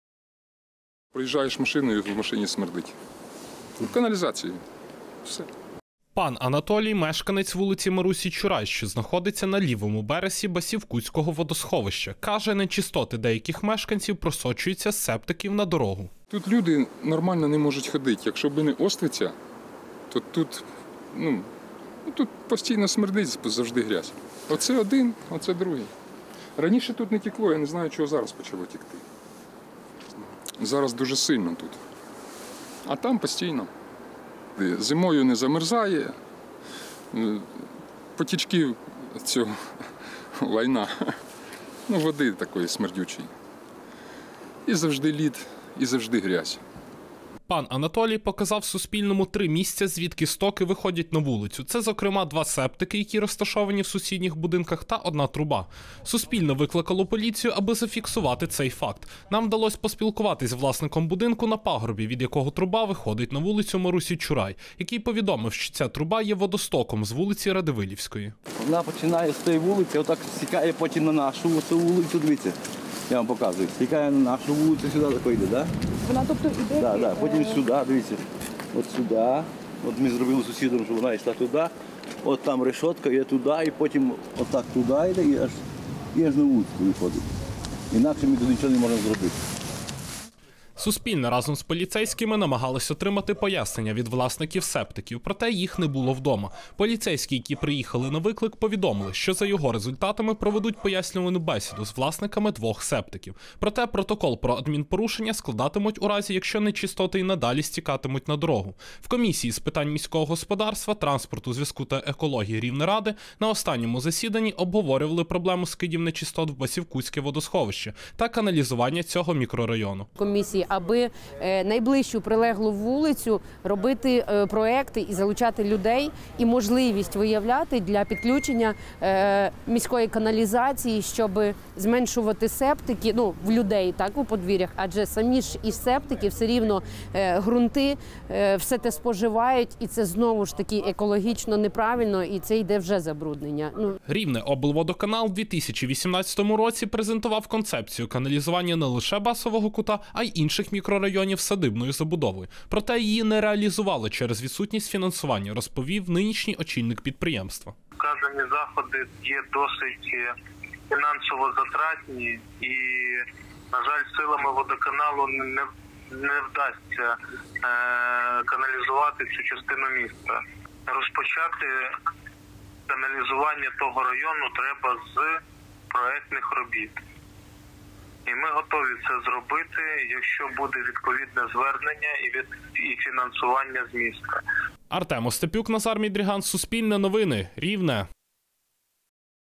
У Рівному стоки з септиків витікають на дорогу
У Рівному на вулиці поблизу Баківкутського водосховища нечистоти з септиків просочуються на проїжджу частину. На це скаржаться місцеві жителі, 21 лютого туди викликали поліцію. Питання каналізування цього мікрорайону обговорювали на профільній комісії Рівнеради, повідомила Суспільному її голова Катерина Марчук. Вже п'ять років розроблена концепція, але її реалізація потребує фінансування, зазначили у Рівнеоблводоканалі.